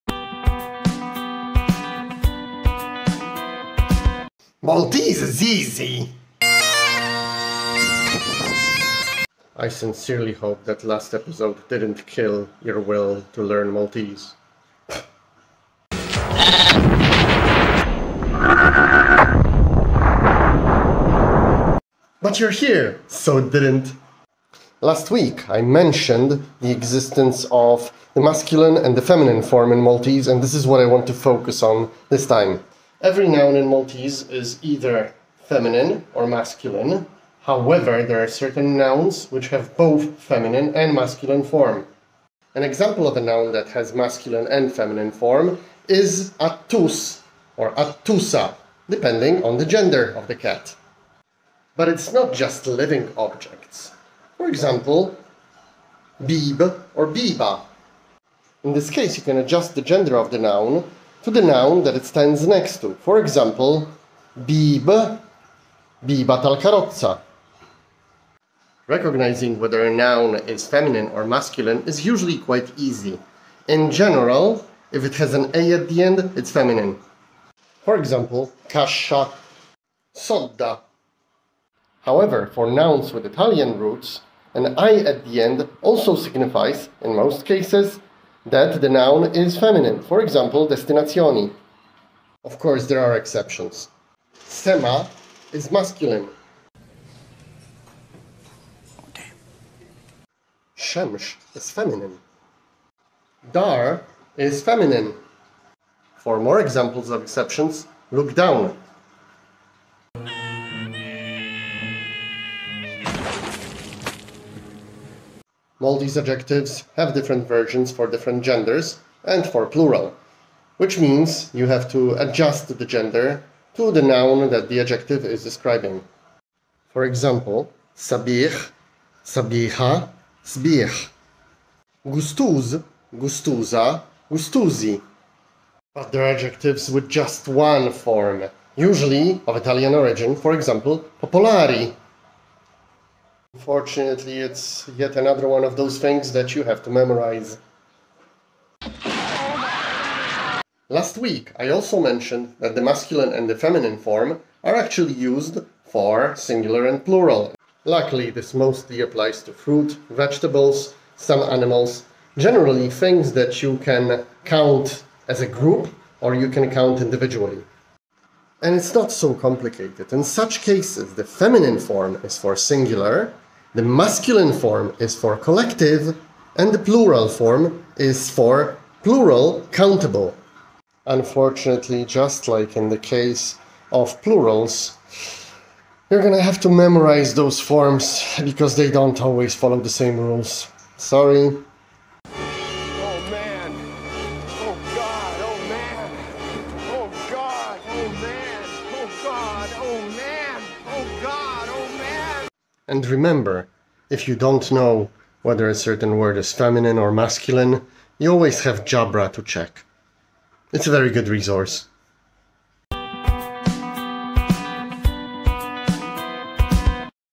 Bits of Maltese – Daqsxejn Malti – Episode 05
An attempt to explain the Maltese language in small bits (so that you don’t choke).
In this episode: genders (masculine and feminine forms)

Exceptions promised in the video:
Masculine not feminine: sema, ilma, papa
Feminine not masculine: xemx, dar, triq, omm, oħt, qalb